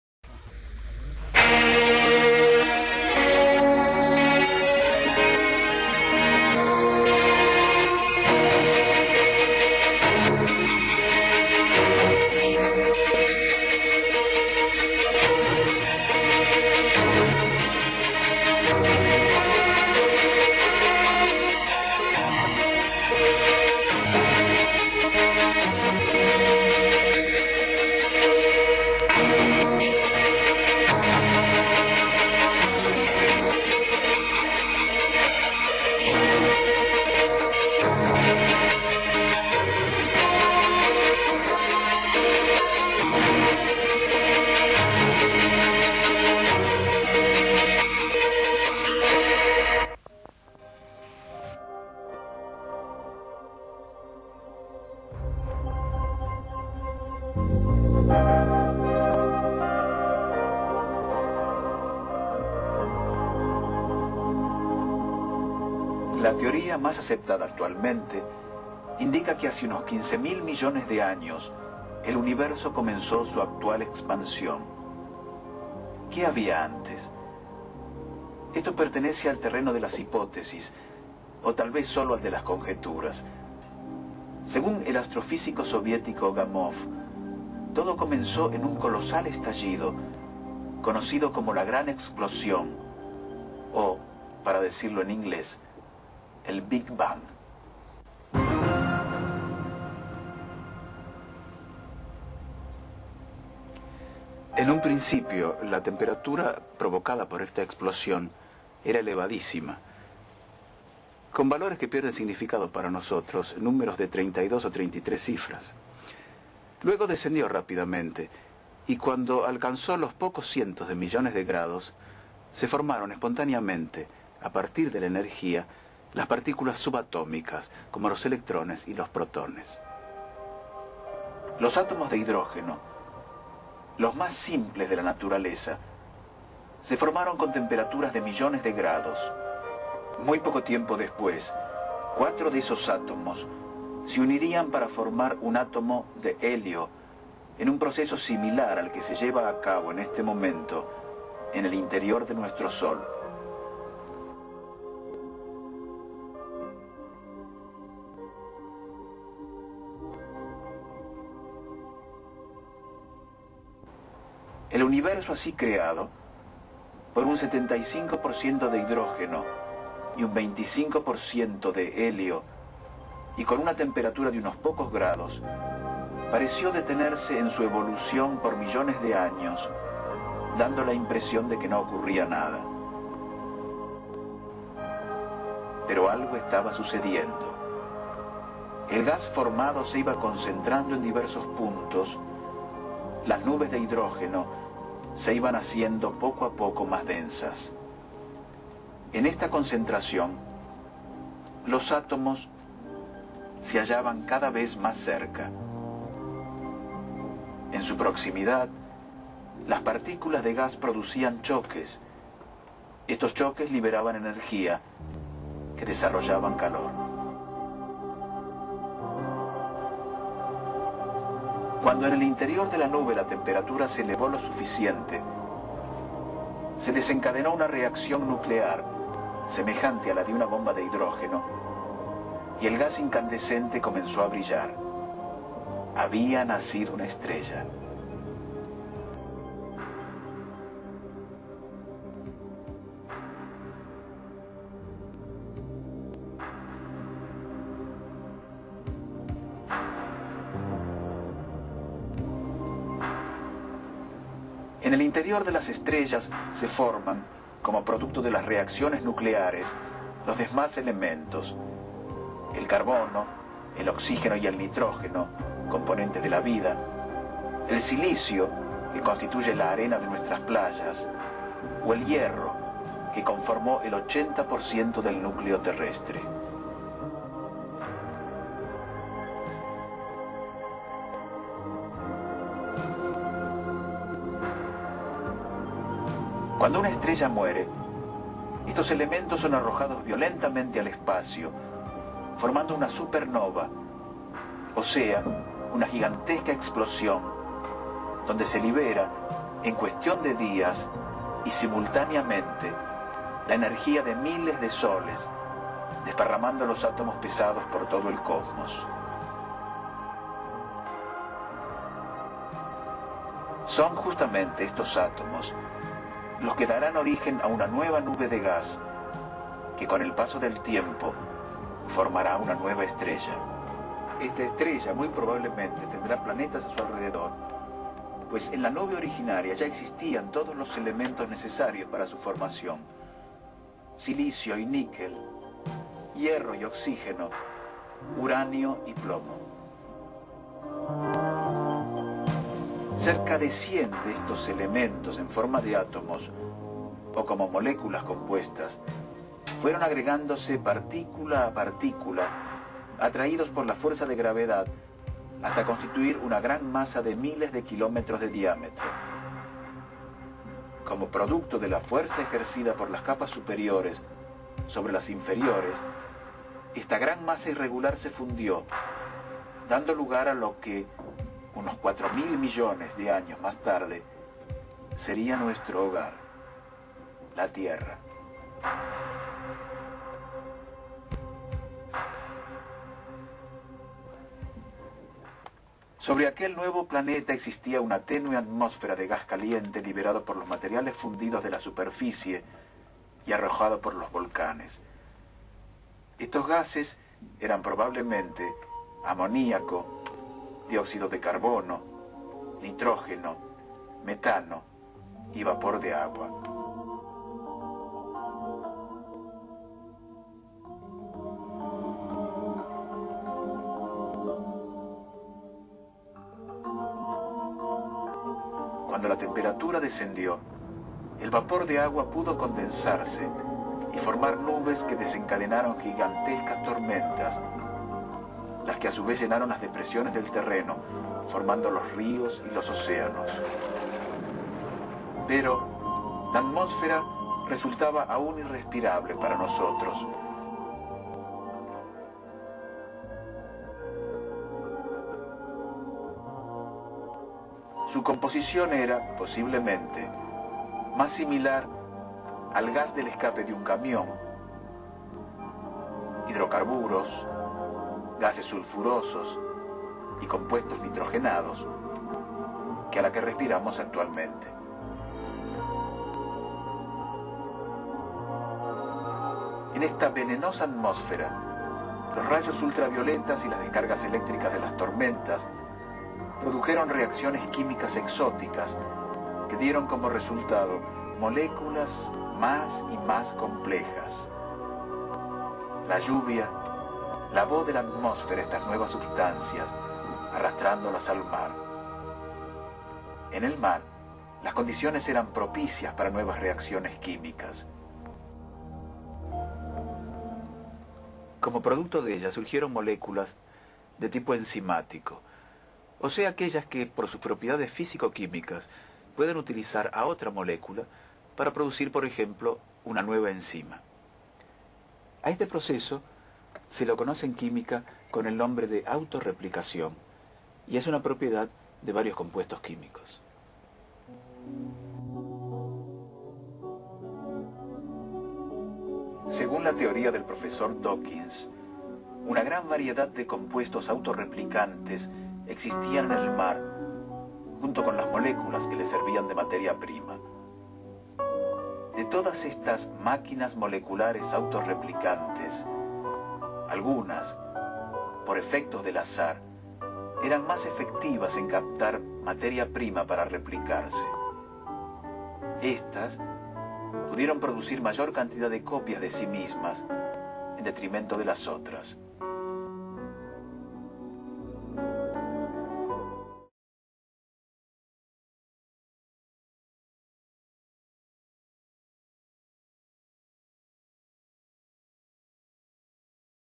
Moleculas exitosas (parte 1)
Breve descripción de la evolución molecular y biológica. (Publico en general)